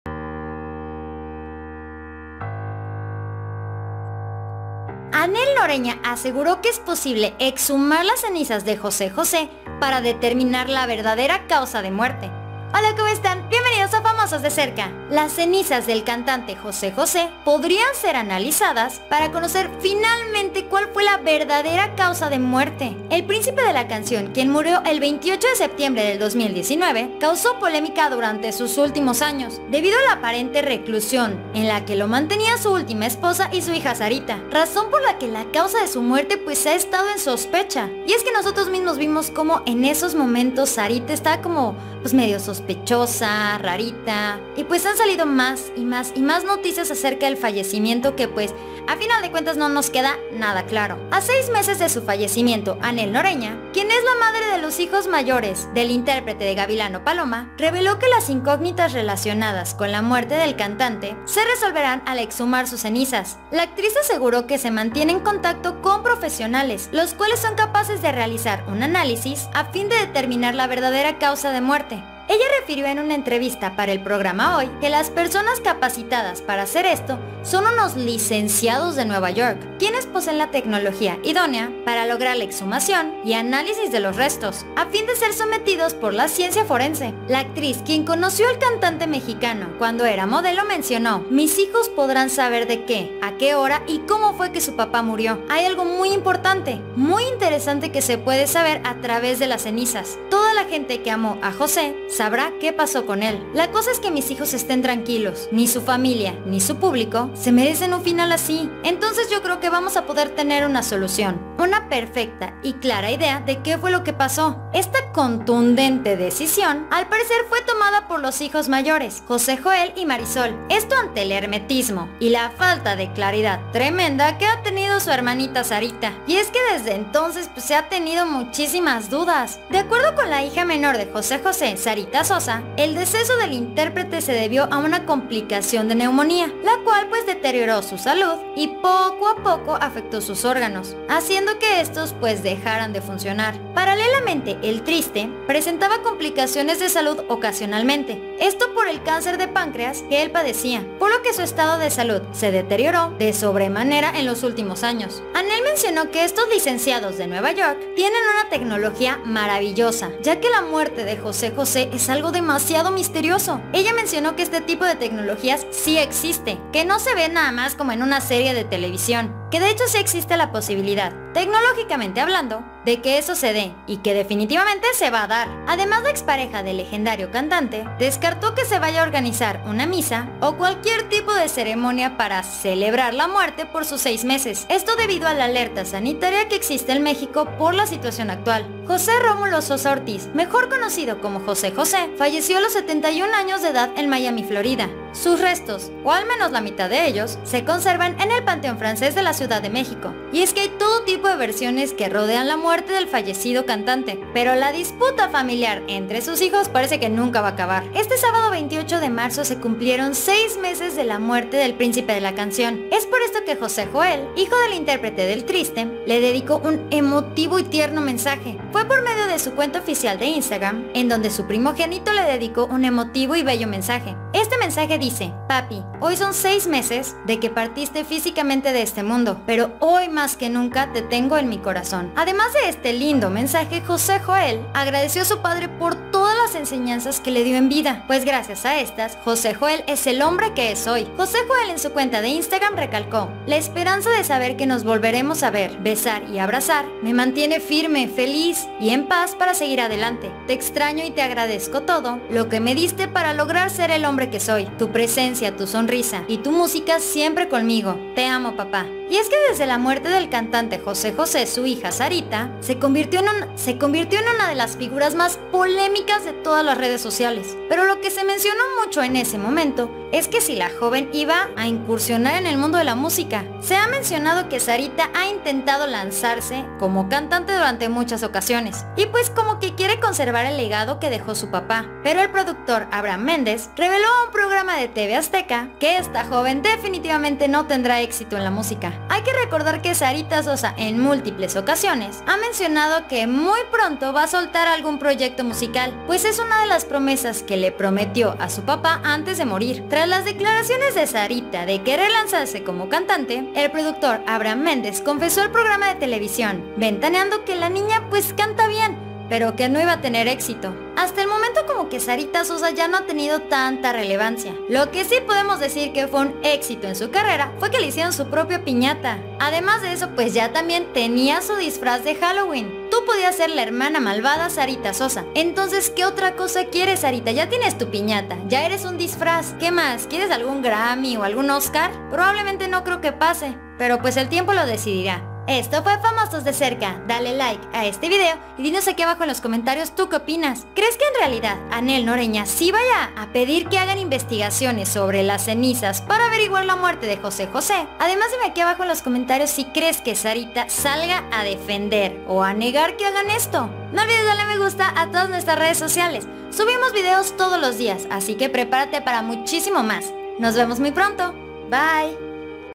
⛔ ¡ Hijos de José José siguen en busca de la verdad sobre el Falle-c¡m!ent0 de su Papa !
La Justicia por fin llegara, por la desconocida Perdida del príncipe de la canción, Hijos de José José no descansaran hasta encontrar la verdad.

 BIENVENIDOS!!!